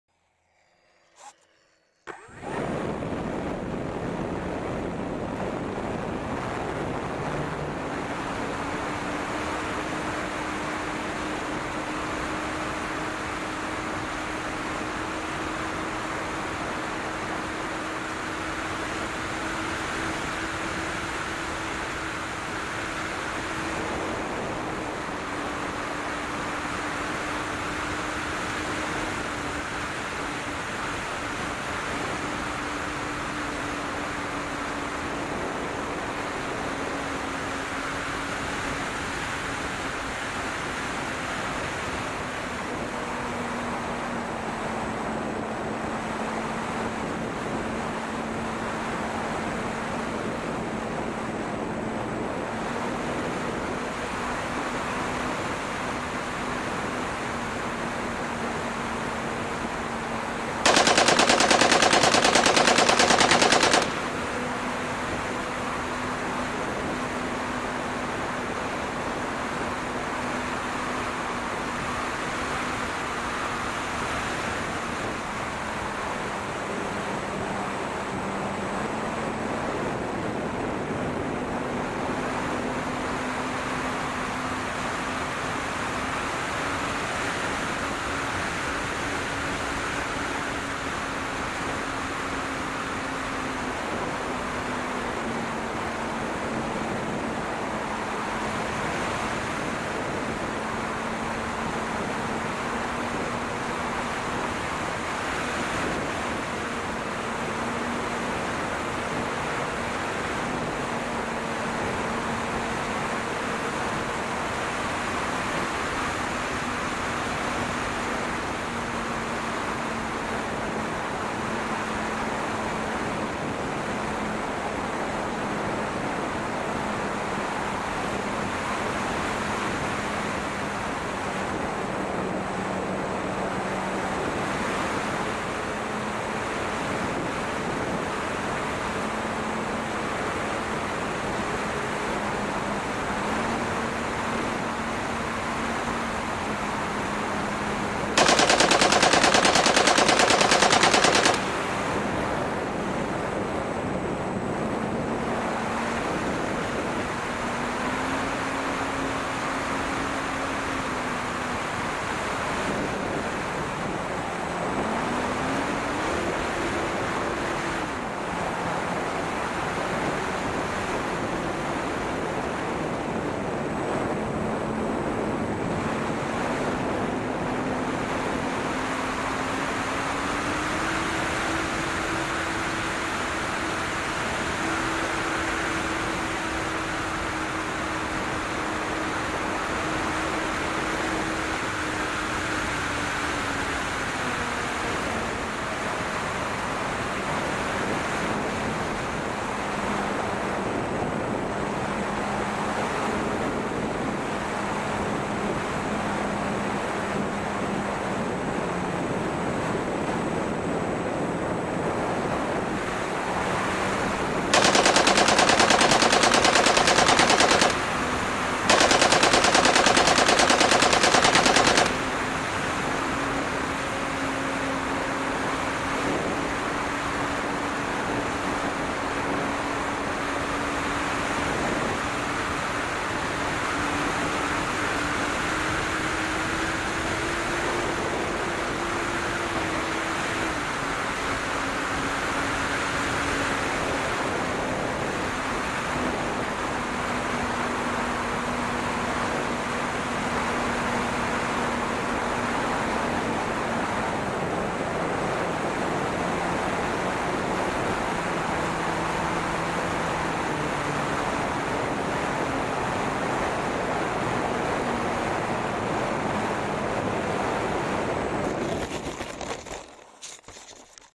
EFlite P-47 Hunting a Stuka, 2017.02.26
When a Stuka came to bomb the airfield, my P-47 sortied to intercept.